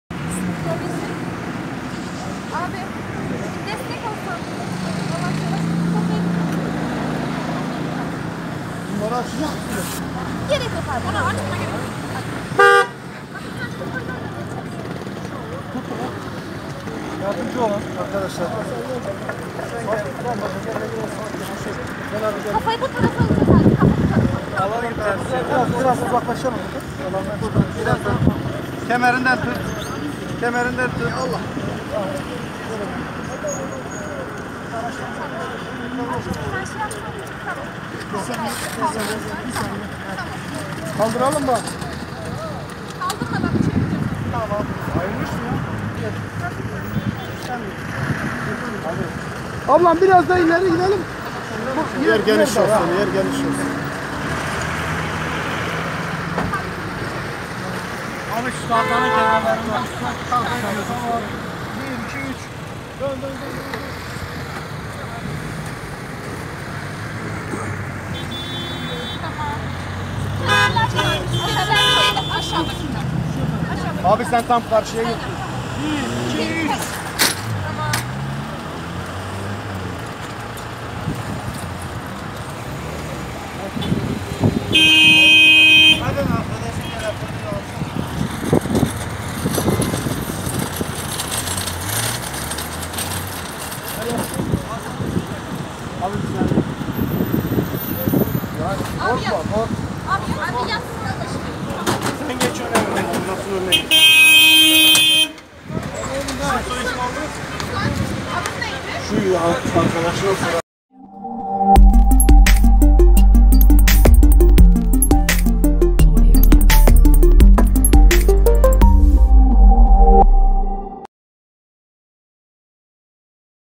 Trafik ışıklarında kaza: Yaralı var!
Akçakale Caddesinde meydana gelen trafik kazasında bir otomobil, arkadan kamyonete çarptı. Kazada otomobil sürücüsü yaralandı.